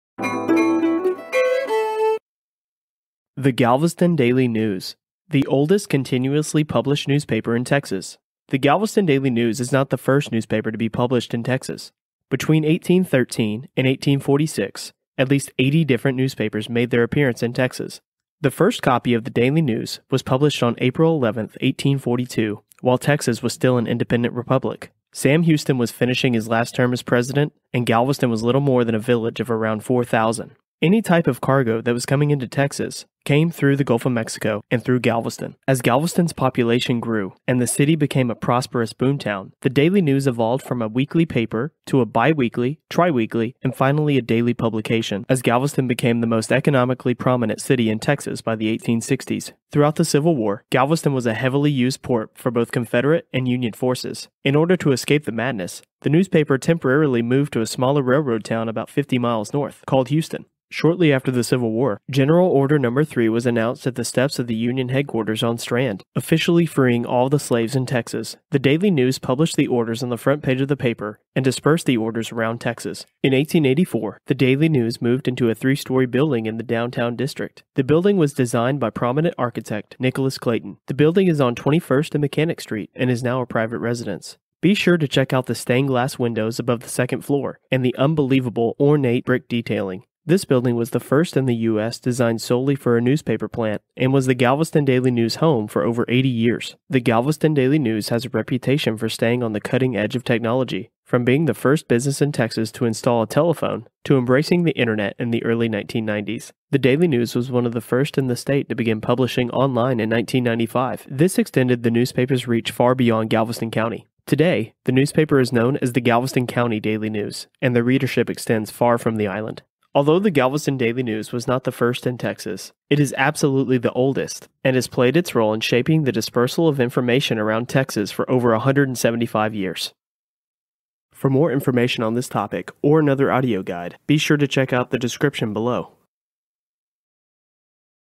The Galveston Daily News - Galveston Unscripted - Free Audio tour of Galveston, Texas
Transcript:
The Galveston Daily News. The oldest continuously published newspaper in Texas. The Galveston daily news is not the first newspaper to be published in Texas. Between 1813 and 1846 at least eighty different newspapers made their appearance in Texas. The first copy of The Daily News was published on April 11, 1842 While Texas was an independent republic. Sam Houston was finishing his last term as president. Galveston was little more than a  village of around 4,000 and any type of cargo that came into Texas from the Gulf of Mexico came through Galveston.As Galveston’s population grew and the city became a prosperous boom town, the Daily News evolved from a weekly paper, to bi-weekly, tri-weekly and finally a daily publication as Galveston became the most populous and economically prominent city in Texas by the 1860s.Throughout the Civil War, Galveston was a heavily used port city for both Confederate and Union forces, in order to escape the madness, the newspaper temporarily moved to a smaller railroad town about 50 miles north, called Houston.After the Civil War, general Granger’s general order number 3 was announced at the steps of the Union headquarters, officially setting all slaves in Texas free. The daily news published the orders on the front page of the paper and dispersed the orders around Texas.In 1884, the Daily news moved into a 3 story building in the downtown district, the building was designed by prominent architect, Nicholas Clayton. The building is on 21st and Mechanic street and is now a private residence. Be sure to check out the stained glass windows above the 2nd floor, and the unbelievable ornate brick detailing.  This building was the first in the United States designed solely for a newspaper plant and was the Galveston daily new’s home for over 80 years. first business in Texas to install a telephone to embracing the Internet in early 1990s.The Daily News was one of the first in the state to begin publishing online in 1995 to extend the newspaper's reach far beyond Galveston County.Today, the newspaper is known as The Galveston County Daily News and the readership extends far from the island. Although the The Galveston Daily News was not the first in Texas, it is absolutely the oldest and has played it’s role in shaping the dispersal of information around Texas for over 175 years.